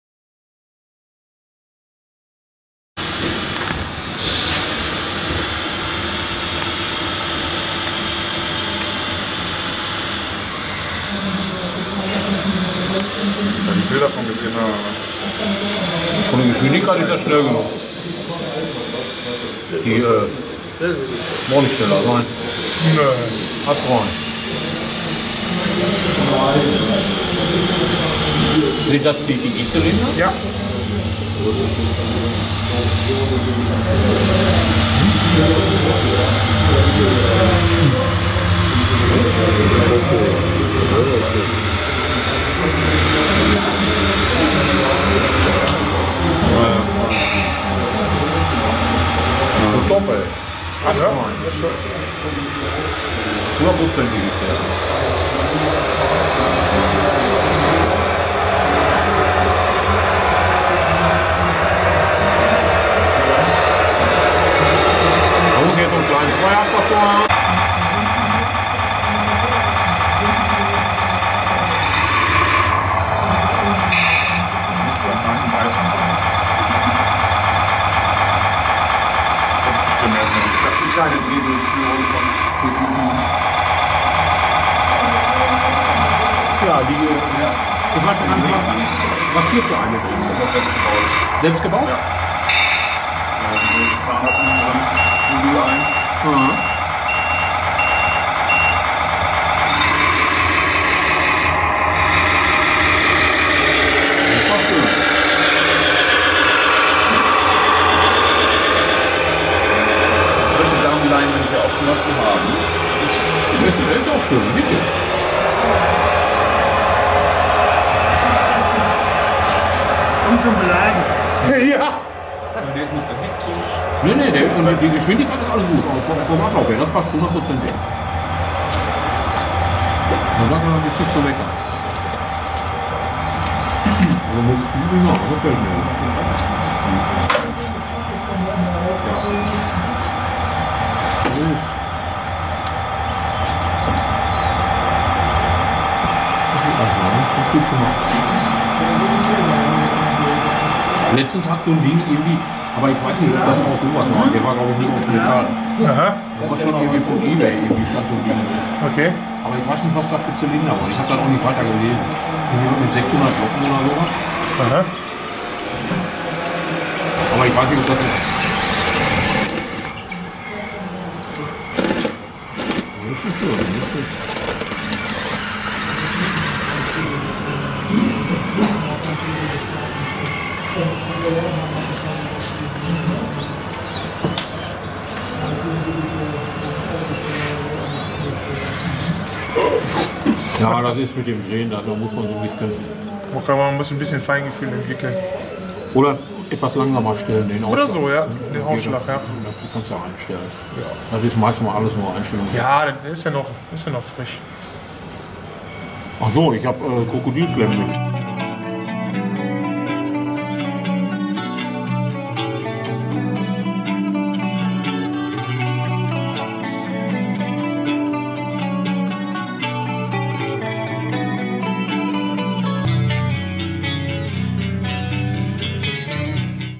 Komatsu PC 88 working at the embankment
Komatsu PC 88 working at the embankment. What a great model!